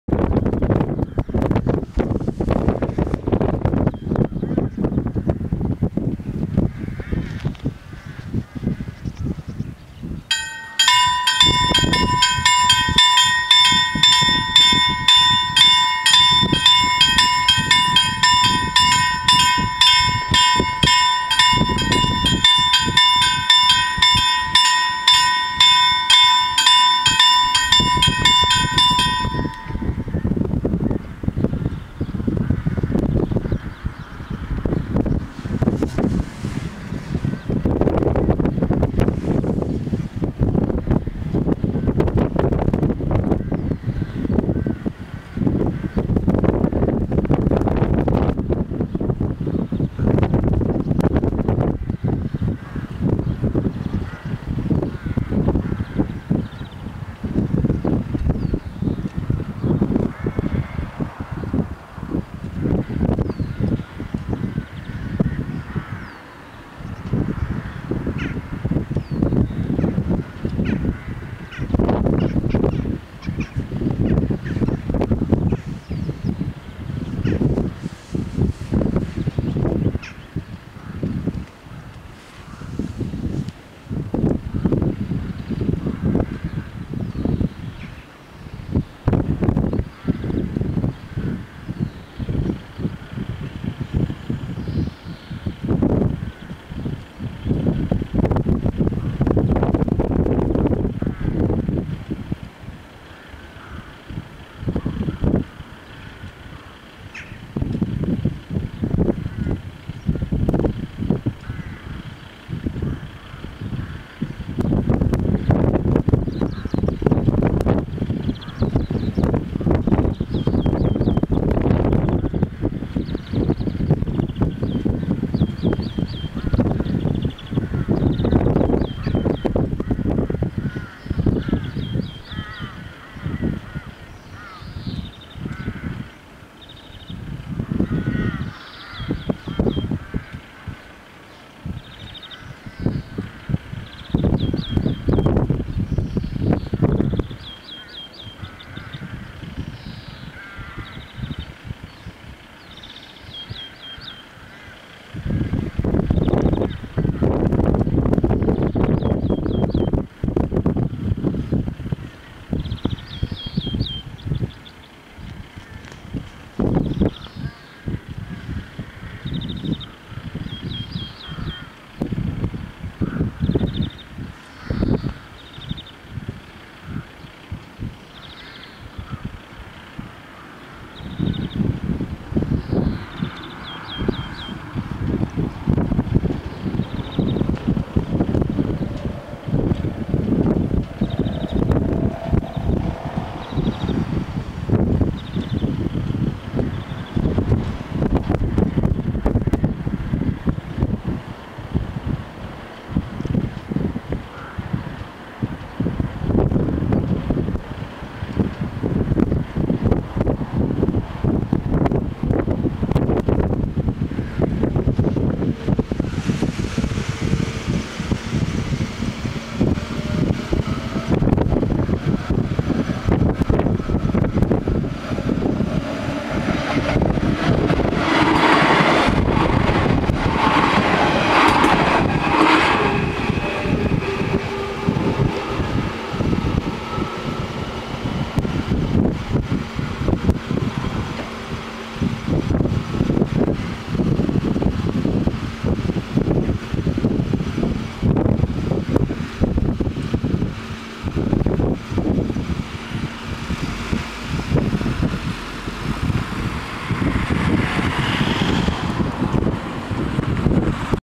Spoorwegovergang Gärsnäs (S) // Railroad crossing // Järnvägsövergång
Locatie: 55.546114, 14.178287
Traject: Simrishamn - Malmö
Rode lichten: 10
Witte lichten: 2
Bellen: 2
Bomen: 4
Andreaskruisen: 5
Passeren:
- X61 als Pågatåg Simrishamn → Helsingborg